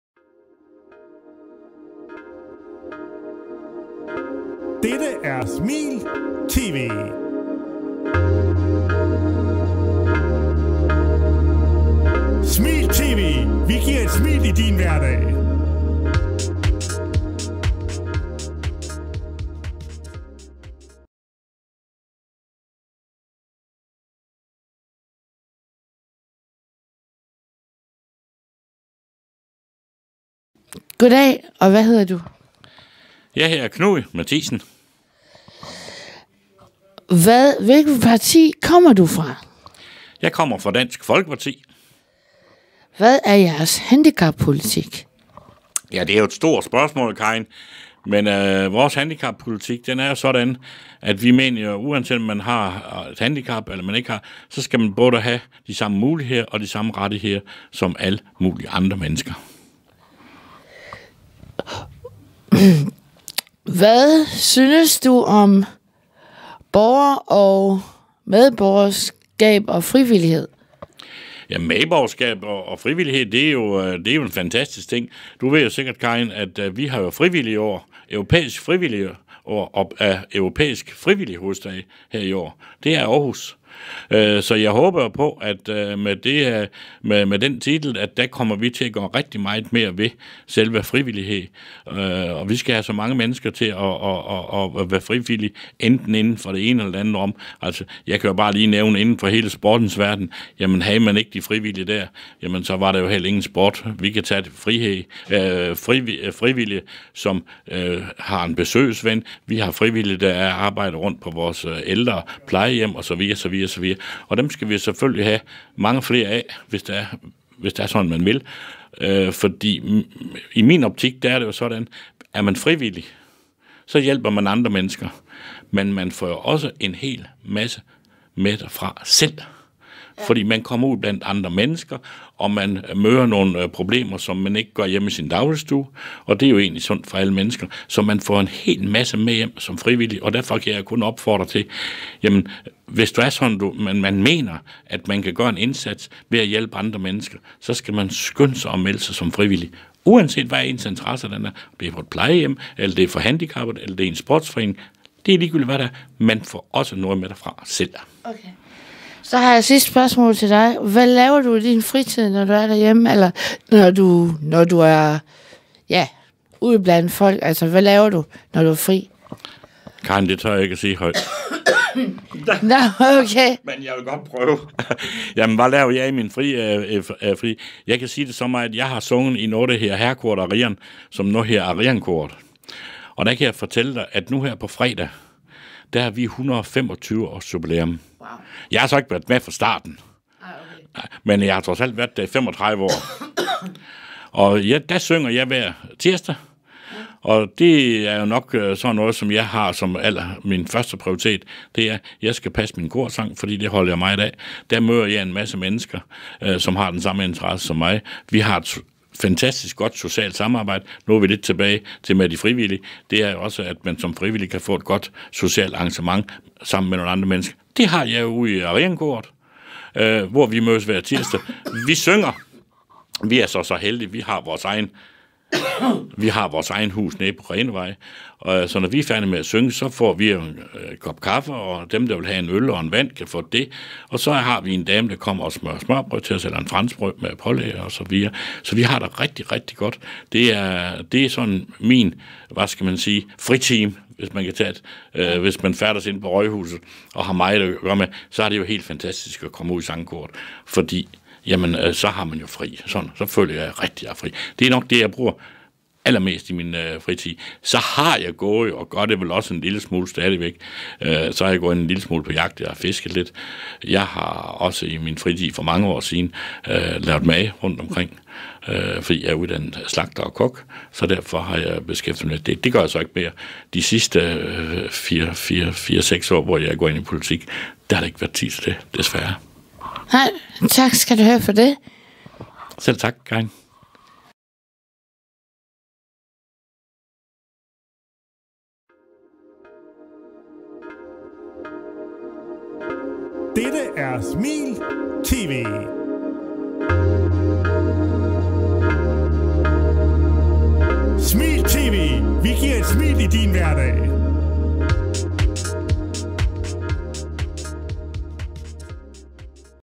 Karin Elgård fra Smil TV Interviewer medlem af Aarhus Byråd Dansk Folkeparti Knud N Mathi
Karin Elgård Jørgensen fra Smil TV Interviewer medlem af Aarhus Byråd Dansk Folkeparti Knud N. Mathiesen (DF).

Tak til Knud N. Mathiesen (DF) for at du ville stille op til et interview.

Interview:
Jounalist
Karin Elgård Jørgensen.

Kamra og Teknik:
Jacob Aakjær Bang.

Redgering:
Jacob Aakjær Bang.

Produceret af:
Smil TV.